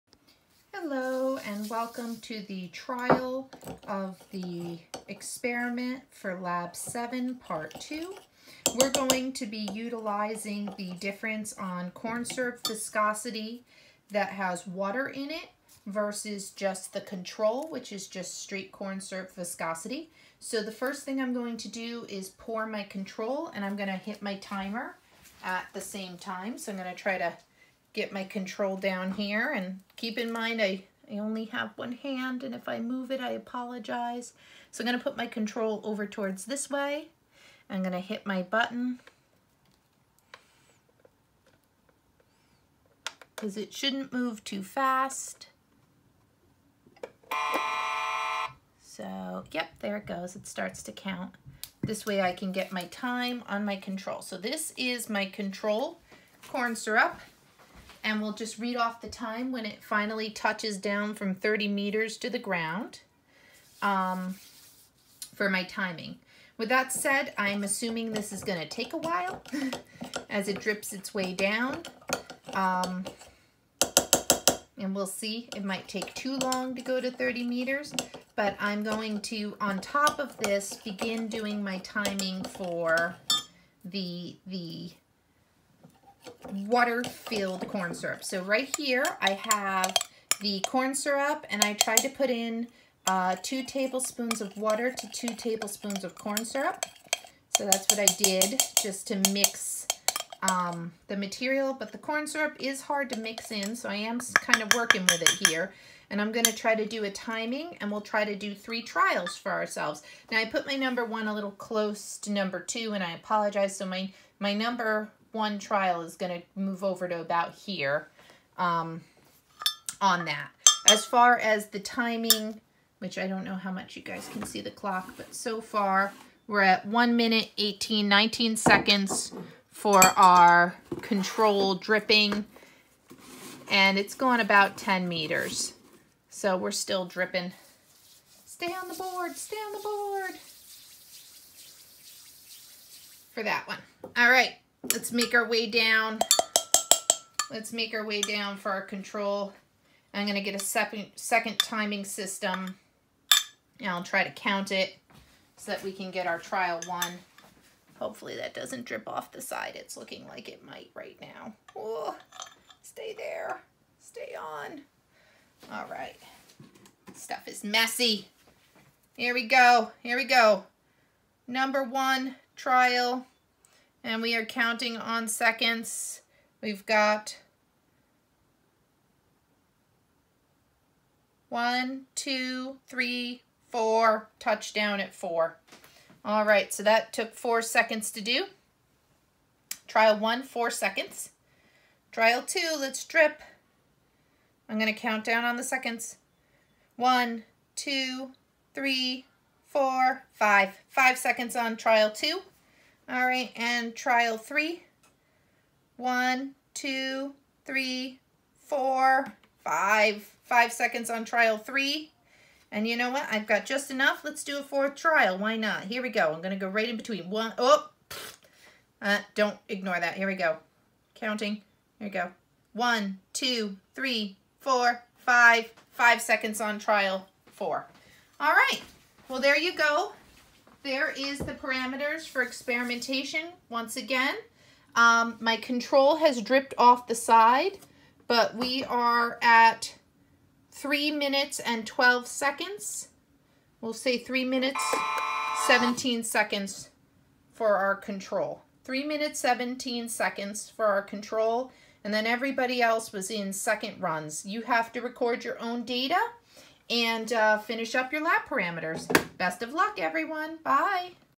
Lab 7 part Viscosity
Length of time to drip 30m on an incline.